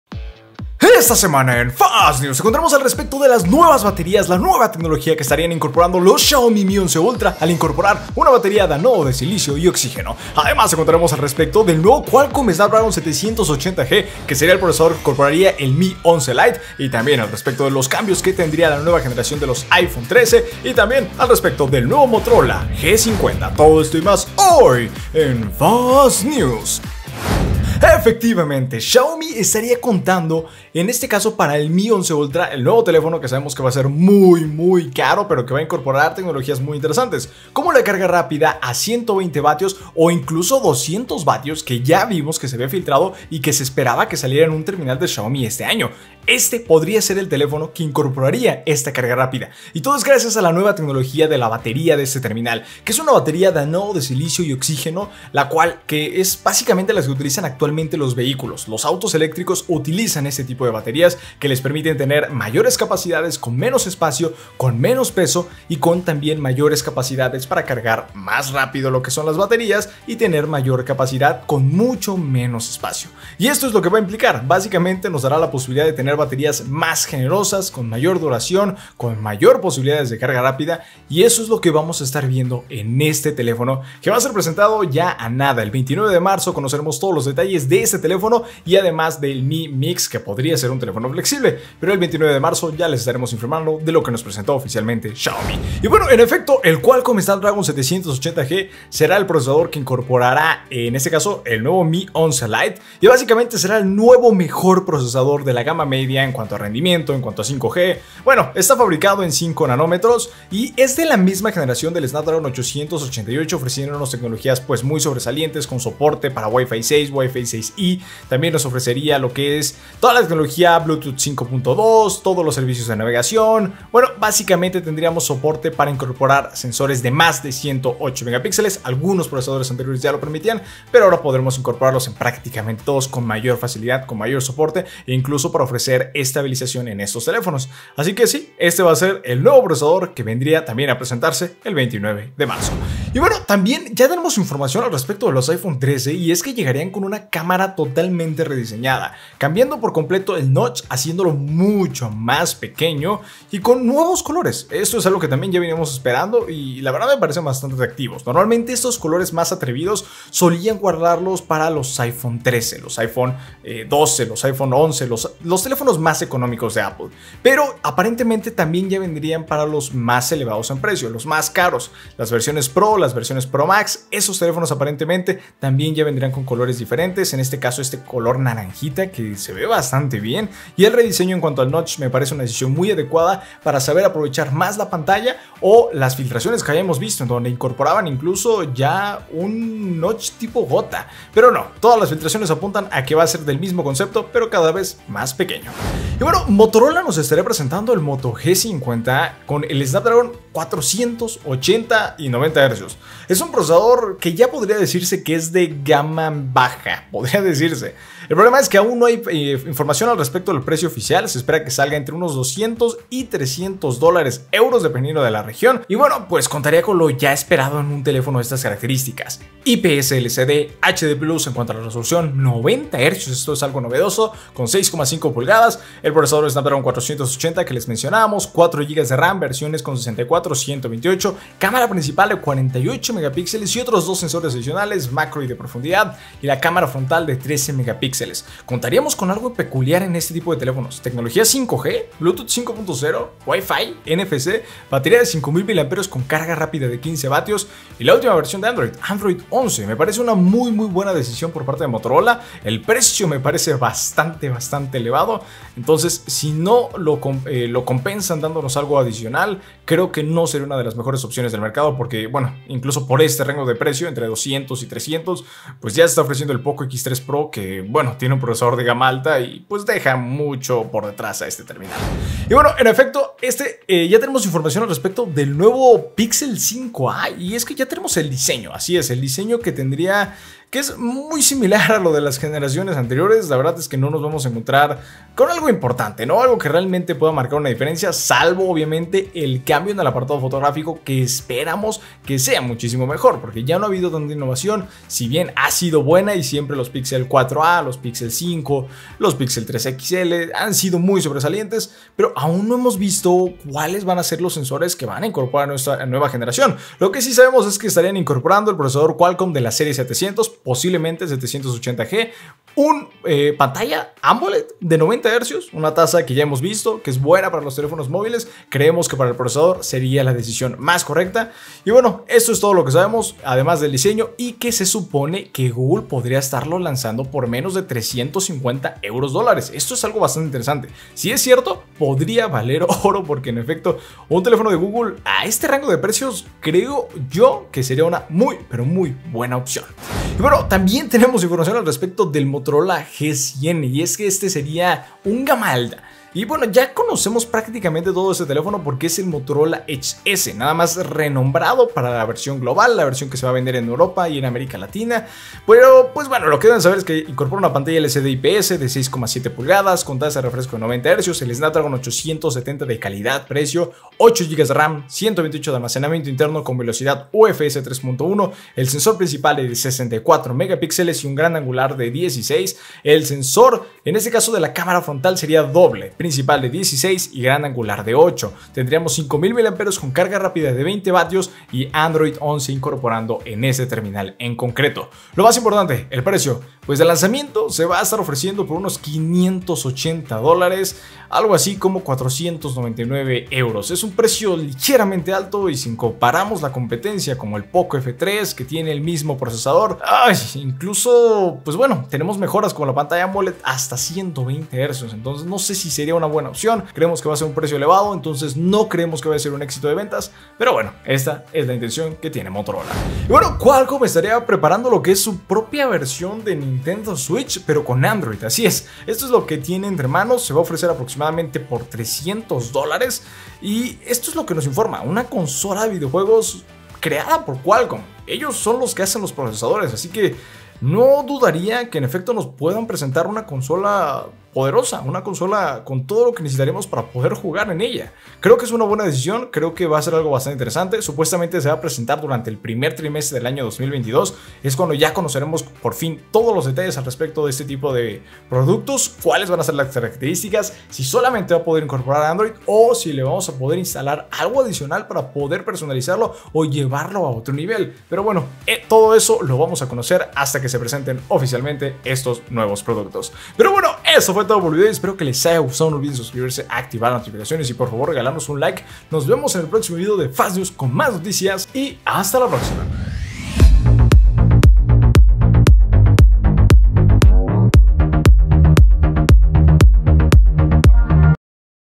XIAOMI MI 11 ULTRA tendrá una BATERÍA NOVEDOSA Y 200W, MI 11 LITE tendrá SNAPDRAGON 780G | FASTNEWS
▶HOY TE MUESTRO EN FASTNEWS
00:00 Introducción
00:27 Xiaomi Mi 11 Ultra Batería Silicio y oxigeno
01:35 Mi 11 lite con Snapdragon 780g
02:22 iPhone 13 con notch pequeño
03:15 Motorola Moto G50
04:30 Google Pixel 5a
05:40 Motorola G100
06:52 Consola tipo Nintendo switch de qualcomm